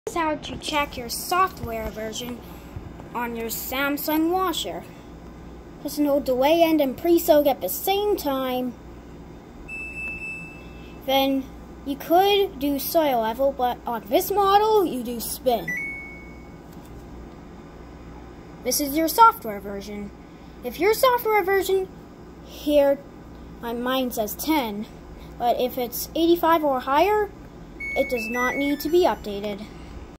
Check software version on washer (Option 2)
Hold Delay End and Pre Soak at the same time. When '00 00' shows, press Soil Level (if 'd' shows up, press Soil Level again, then Spin). The software version is the 2 last digits (in my case, 10). If it is 85 or higher, it may not be updated.